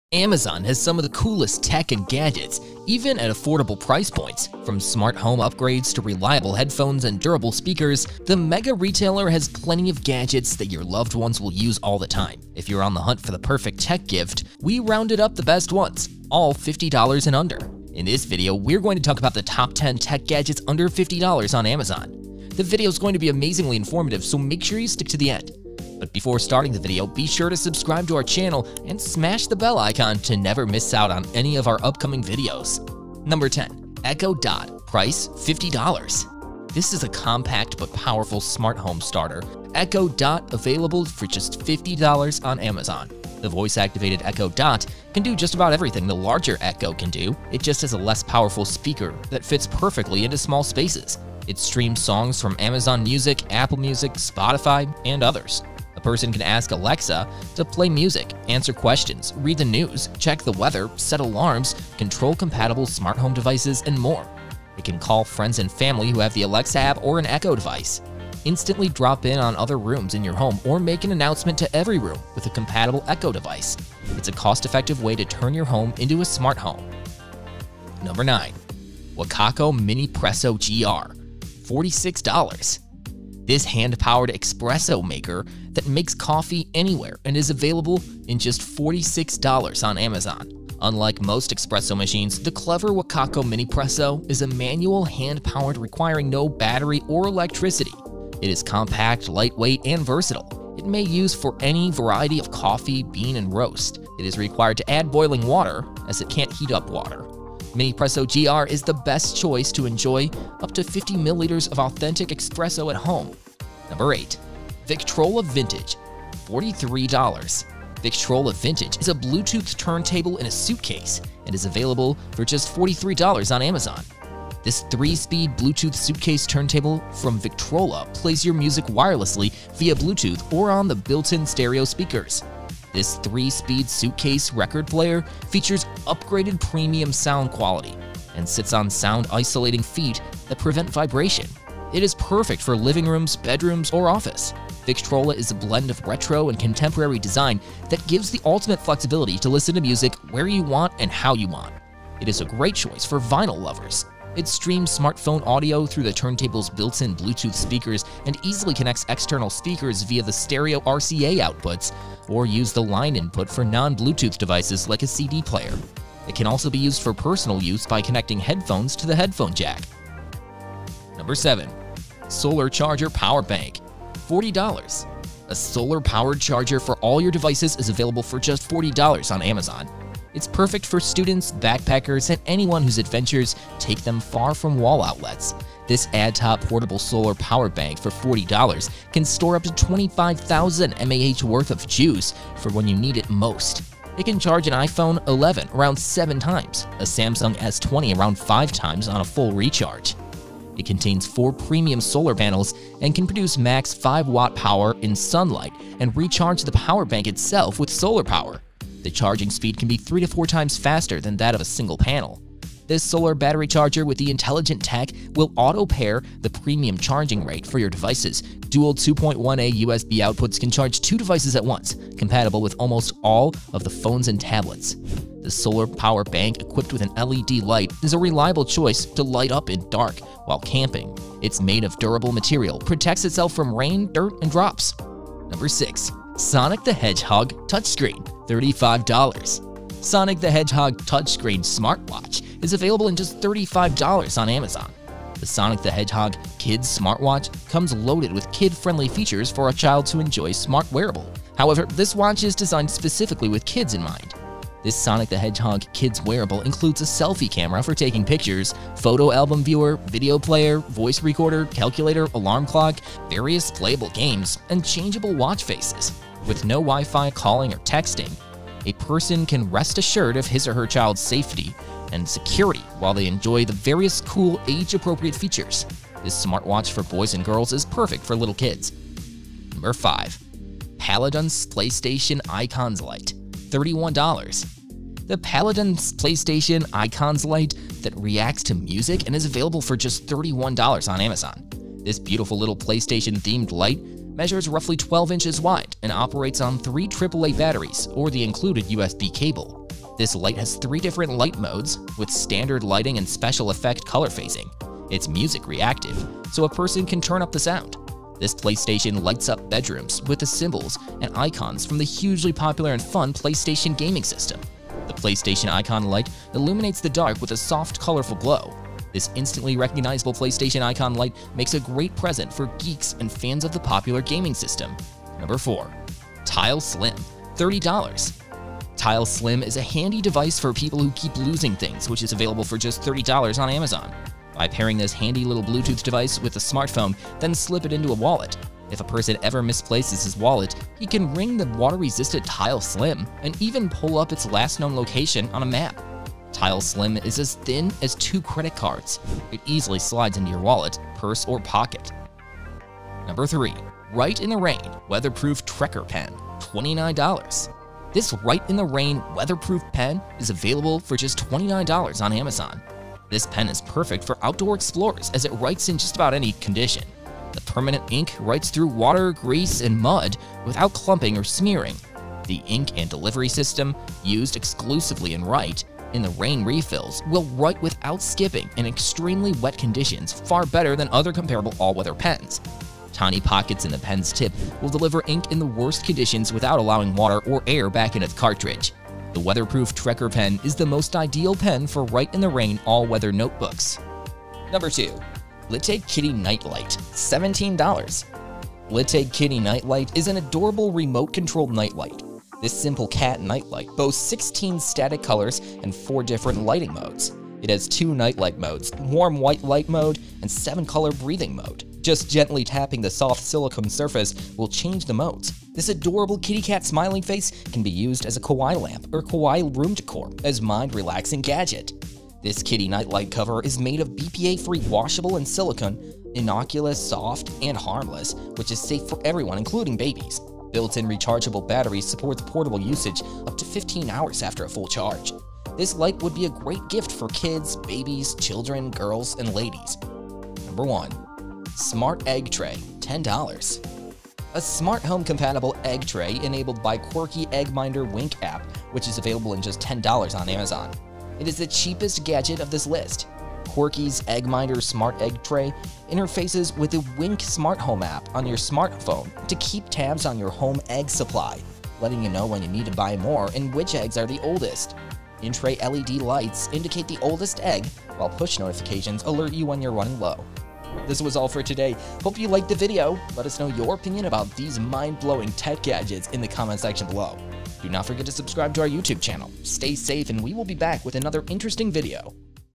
10 Best Gadgets under $50
Amazon has some of the coolest tech and gadgets, even at affordable price points. From smart home upgrades to reliable headphones and durable speakers, the megaretailer has plenty of gadgets that your loved ones will use all the time. If you're on the hunt for the perfect tech gift, we rounded up the best ones, all $50 and under.

Content
0:00 -  Intro

0:32 - Echo Dot

1:18 - WACACO Minipresso GR

1:53 - Victrola Vintage

2:51 - Solar Charger Power Bank

3:55 - Sonic Smartwatch

4:39 - Paladone Playstation Icons Light

5:24 - Tile Slim

5:53 - Weatherproof Pen

6:34 - Kitty Night Light

7:21 - Smart Egg tray